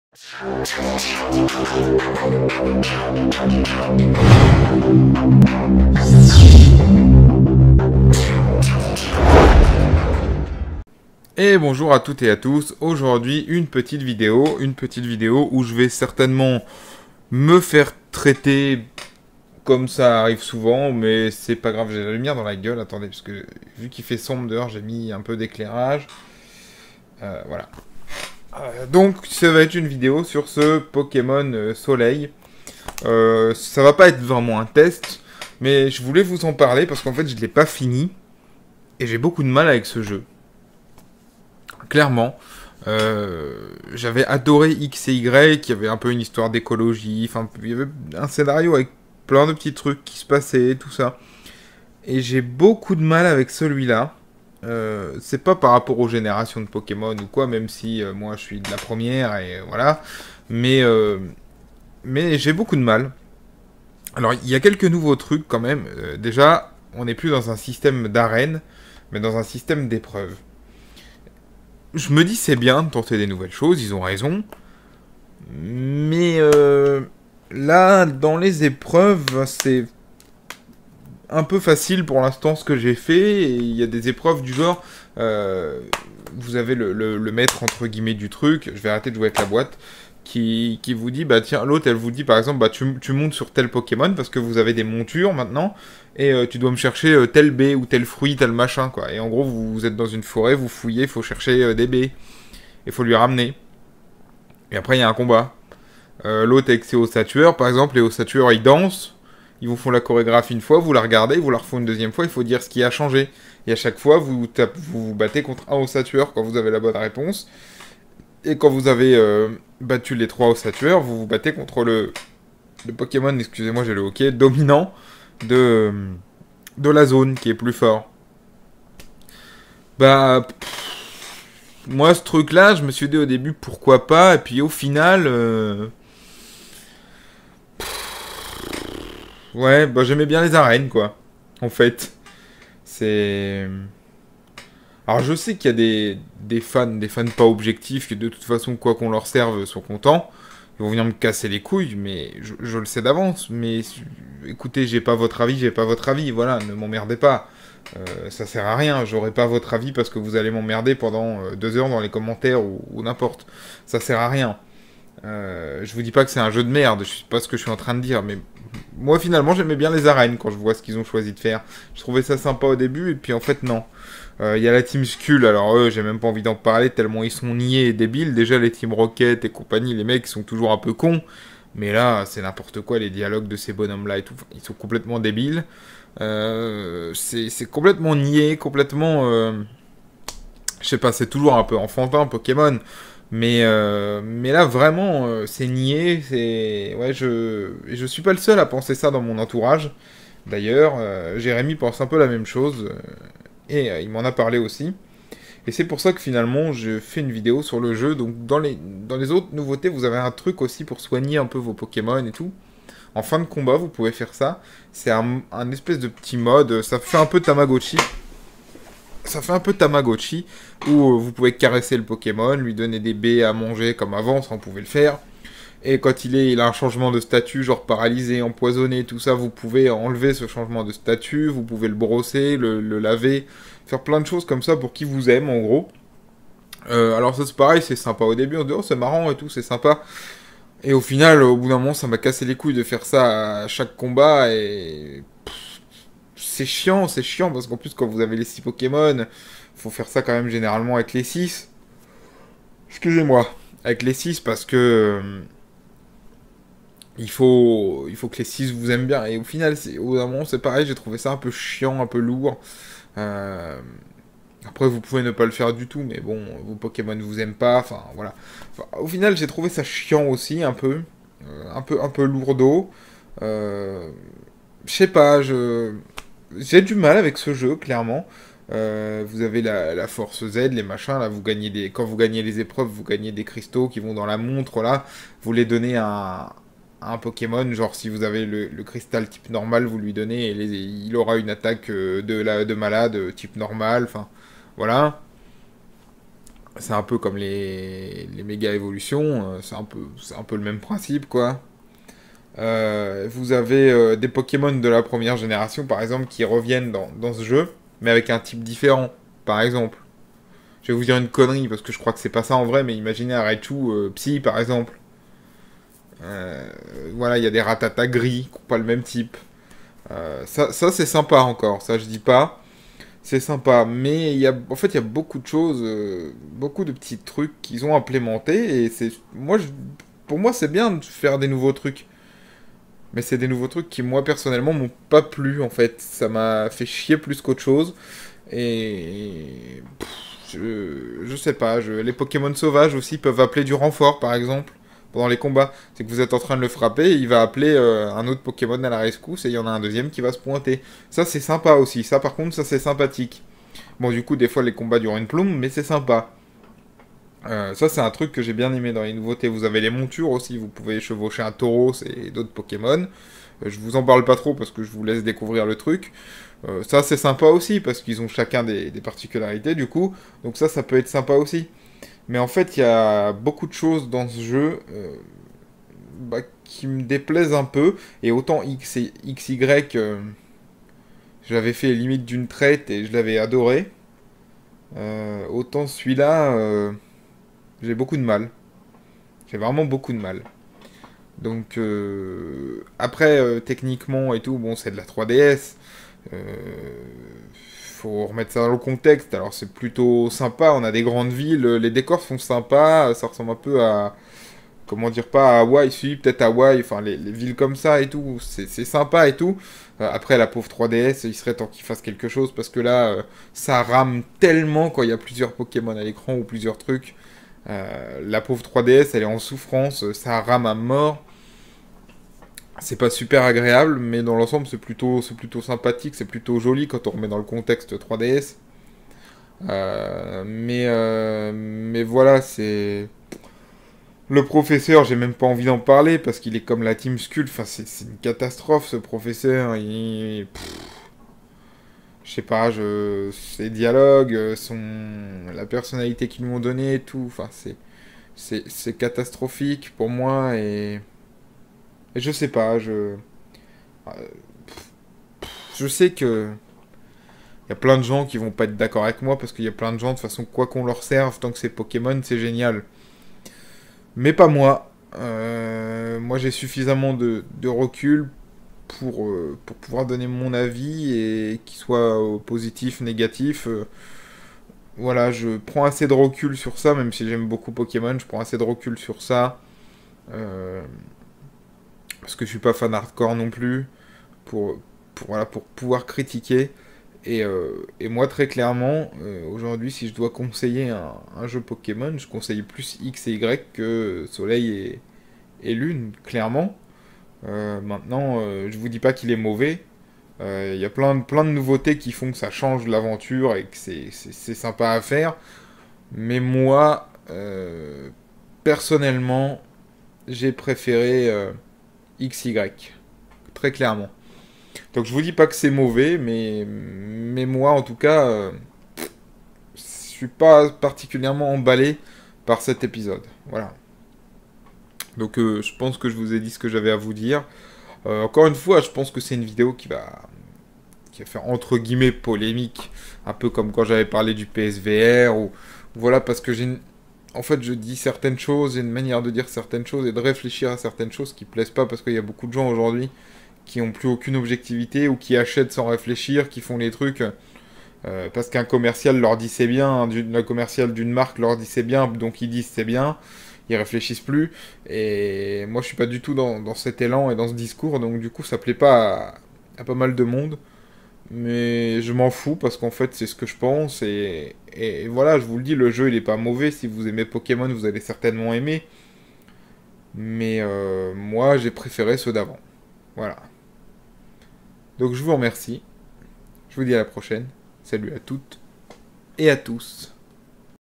POKEMON SOLEIL & LUNE : MON AVIS
Petite vidéo de mon avis sur ce nouveau pokemon a la base j'avais mis des images de jeu mais on ma bloqué la vidéo . n'hesite pas a donner ton avis et partage la vidéo si elle te plait